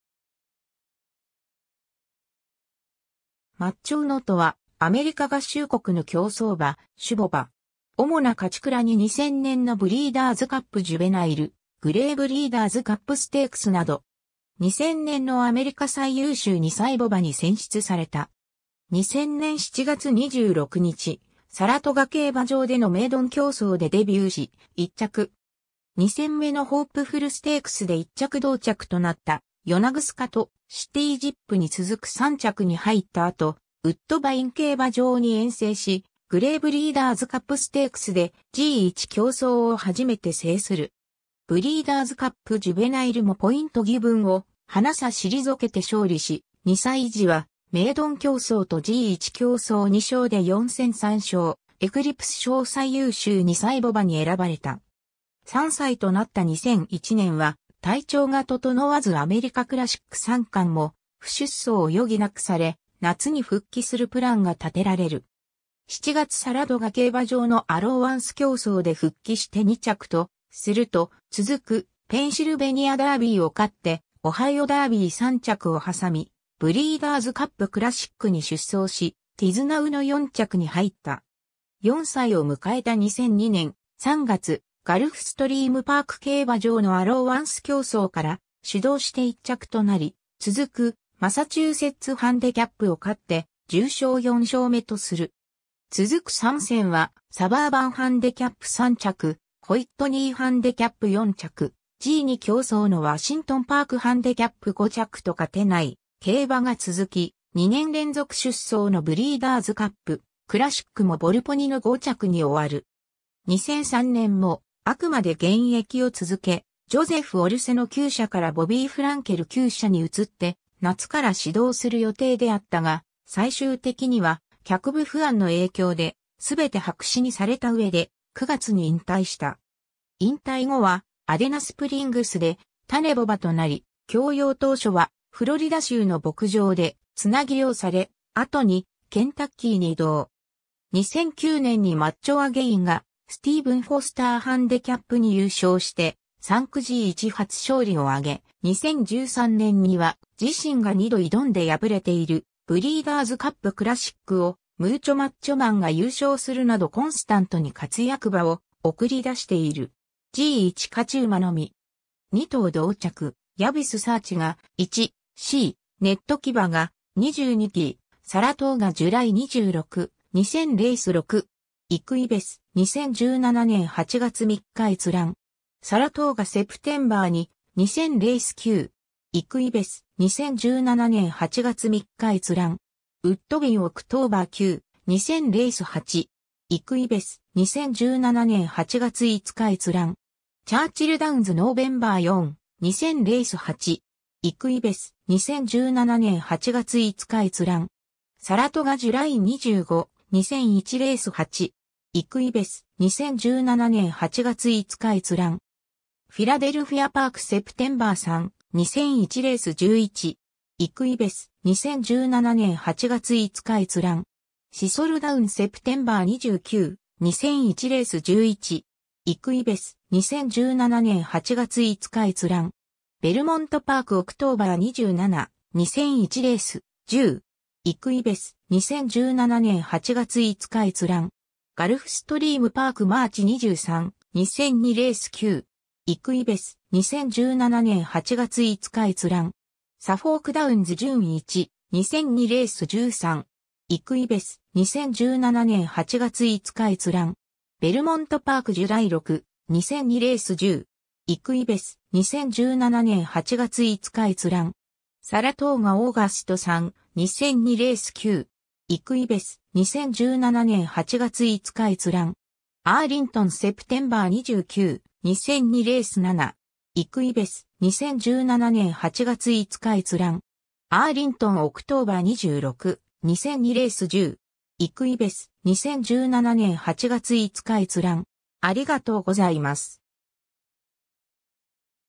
マッチョウノ
マッチョウノ (Macho Uno) とはアメリカ合衆国の競走馬、種牡馬。
主な勝ち鞍に2000年のブリーダーズカップ・ジュヴェナイル、グレイブリーダーズカップステークスなど。
2000年のアメリカ最優秀2歳牡馬に選出された。
 2000年7月26日、サラトガ競馬場でのメイドン競走でデビューし、1着。
2戦目のホープフルステークスで1着同着となったヨナグスカとシティージップに続く3着に入った後ウッドバイン競馬場に遠征し、グレイブリーダーズカップステークスでG1競走を初めて制する。
ブリーダーズカップ・ジュヴェナイルもポイントギヴンをハナ差退けて勝利し、2歳時はメイドン競走とG1競走2勝で4戦3勝、エクリプス賞最優秀2歳牡馬に選ばれた。
 3歳となった2001年は体調が整わずアメリカクラシック三冠も不出走を余儀なくされ、夏に復帰するプランが立てられる。
7月サラトガ競馬場のアローワンス競走で復帰して2着とすると、続くペンシルベニアダービーを勝ってオハイオダービー3着を挟みブリーダーズカップ・クラシックに出走し、ティズナウの4着に入った。
 4歳を迎えた2002年、3月ガルフストリームパーク競馬場のアローワンス競争から始動して1着となり、続くマサチューセッツハンデキャップを勝って重賞4勝目とする。
続く3戦はサバーバンハンデキャップ3着、ホイットニーハンデキャップ4着、G2競走のワシントンパークハンデキャップ5着と勝てない競馬が続き、2年連続出走のブリーダーズカップ・クラシックもヴォルポニの5着に終わる。
2003年もあくまで現役を続け、ジョゼフ・オルセノ厩舎からボビー・フランケル厩舎に移って夏から始動する予定であったが、最終的には脚部不安の影響ですべて白紙にされた上で、9月に引退した。
 引退後はアデナスプリングスで種牡馬となり、供用当初はフロリダ州の牧場で繋養され、後にケンタッキーに移動。
2009年にマッチョアゲインがスティーブンフォスターハンデキャップに優勝して産駒G1初勝利を挙げ、2013年には自身が二度挑んで敗れているブリーダーズカップ・クラシックをムーチョマッチョマンが優勝するなどコンスタントに活躍馬を送り出している。
 G1勝ち馬のみ。
 ^ 2頭同着 ^ JBISサーチが「1-c」、netkeibaが「22-d」 ^ “Salatoga - July 26, 2000 - Race 6 (PDF)”. Equibase. 2017年8月3日閲覧。
 ^ “Salatoga - September 2, 2000 - Race 9 (PDF)”. Equibase. 2017年8月3日閲覧。
 ^ “Woodbine - October 9, 2000 - Race 8 (PDF)”. Equibase. 2017年8月5日閲覧。
 ^ “Churchill Downs - November 4, 2000 - Race 8 (PDF)”. Equibase. 2017年8月5日閲覧。
 ^ “Saratoga - July 25, 2001 - Race 8 (PDF)”. Equibase. 2017年8月5日閲覧。
 ^ “Philadelphia Park - September 3, 2001 - Race 11 (PDF)”. Equibase. 2017年8月5日閲覧。
 ^ “Thistledown - September 29, 2001 - Race 11 (PDF)”. Equibase. 2017年8月5日閲覧。
 ^ “Belmont Park - October 27, 2001 - Race 10 (PDF)”. Equibase. 2017年8月5日閲覧。
 ^ “GulfStream Park - March 23, 2002 - Race 9 (PDF)”. Equibase. 2017年8月5日閲覧。
 ^ “Suffolk Downs - June 1, 2002 - Race 13 (PDF)”. Equibase. 2017年8月5日閲覧。
 ^ “Belmont Park - July 6, 2002 - Race 10 (PDF)”. Equibase. 2017年8月5日閲覧。
 ^ “Salatoga - August 3, 2002 - Race 9 (PDF)”. Equ...